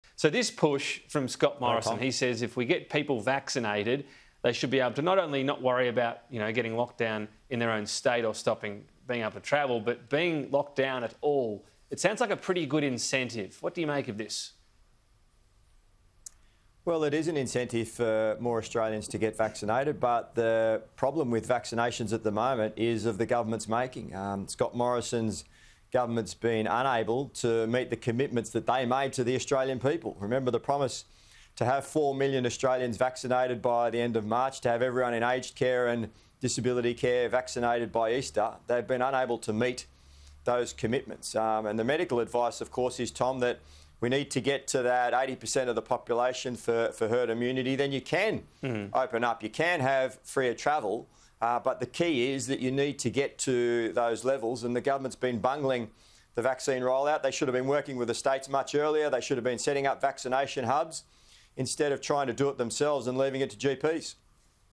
Vaccine rollout problems are ‘of the government’s making'
Labor MP Matt Thistlethwaite says while access to greater freedoms is a good incentive to encourage vaccine uptake the problems with the rollout are “of the government’s making”.

“Scott Morrison’s government’s been unable to meet the commitments that they made to the Australian people,” he said.

“Remember the promise to have four million Australians vaccinated by the end of March, to have everyone in aged care and disability care vaccinated by Easter - they’ve been unable to meet those commitments.

“The medical advice of course is that we need to get to about 80 per cent of the population for herd immunity, then you can open up, you can have free travel”.